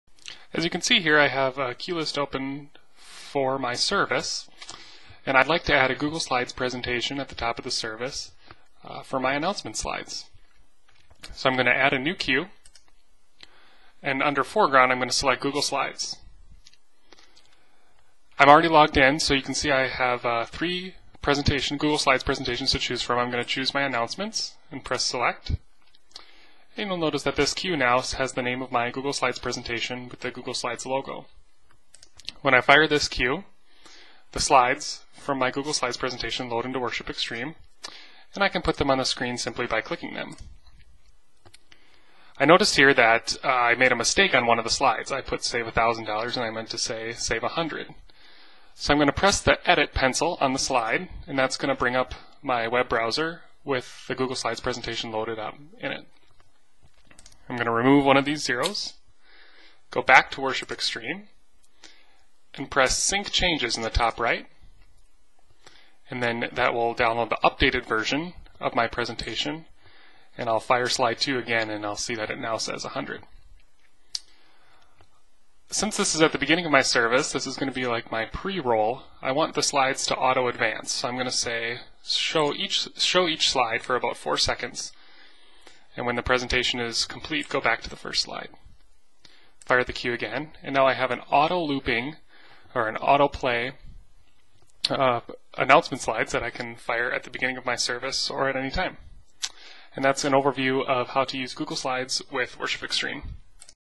Using Google Slides in Presenter
This video uses an old version of WorshipTools' Presenter app, from when it was formerly known as Worship Extreme. The functionality has largely remained the same, but the appearance of the app is different. We will be working on updating all these videos soon.